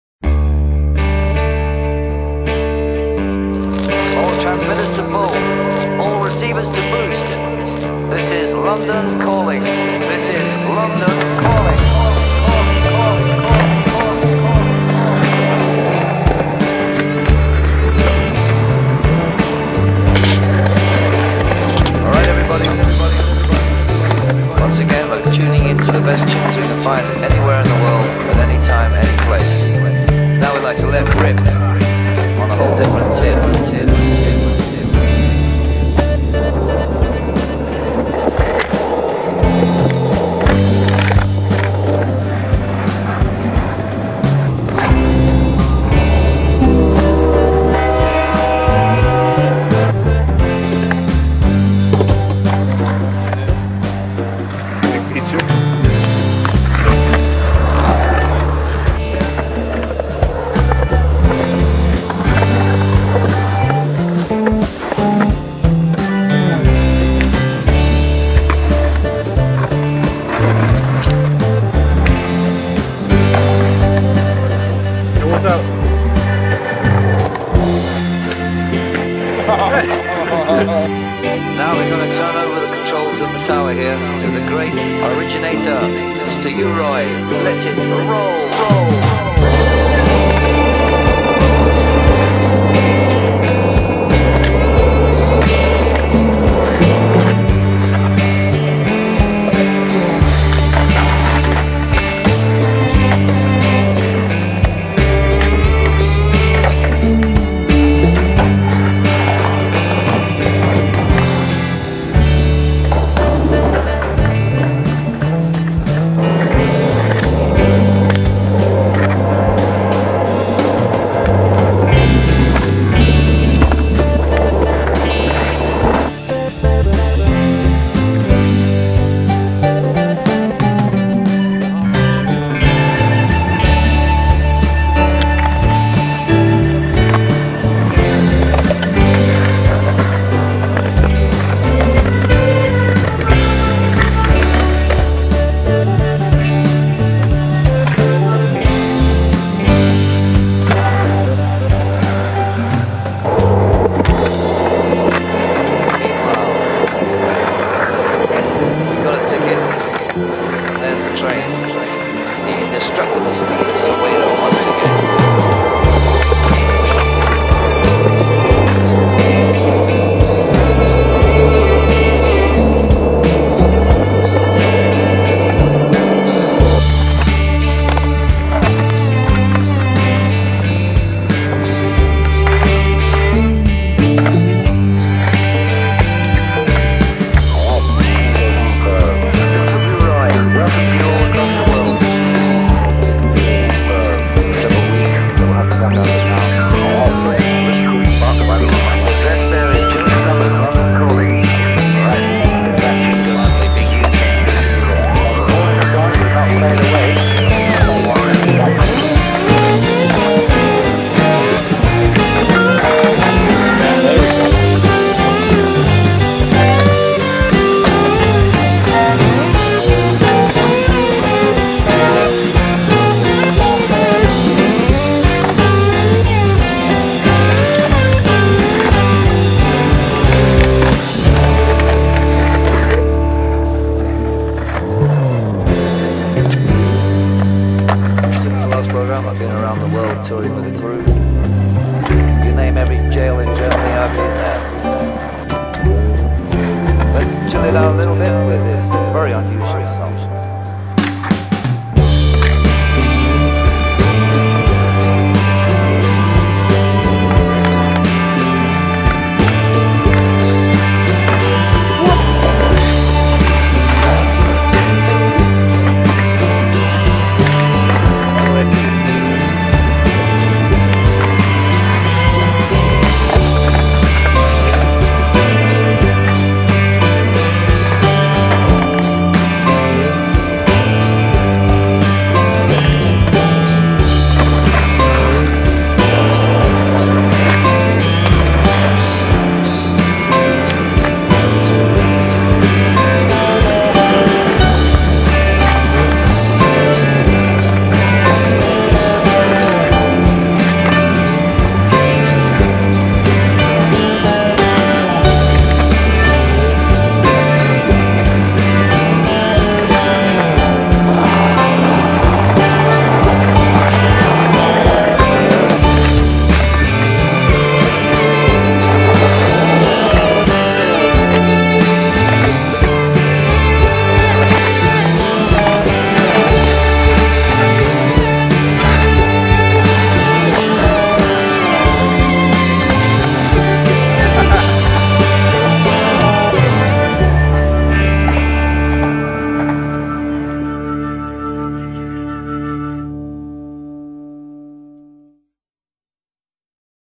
Ryan Sweeney In Enjoy The Summer
Hope you enjoyed the Summer as much as we did. Heres a short Summer bit our buddy Ryan edited himself.